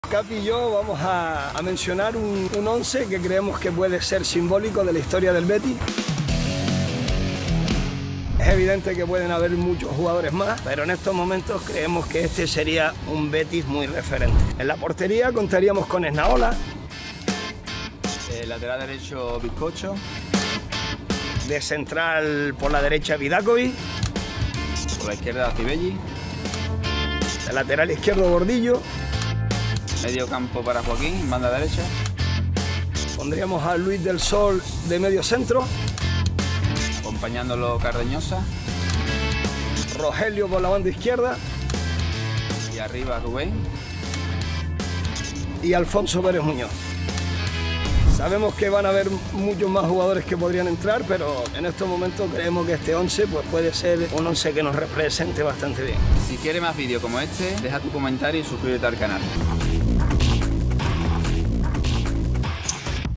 11 Histórico ideal | Jesús Capitán y Alexis Trujillo | Real Betis Balompié
¿Cuál es el "11 ideal" histórico del Real Betis Balompié para Jesús Capitán y Alexis Trujillo? ¡No te pierdas este vídeo!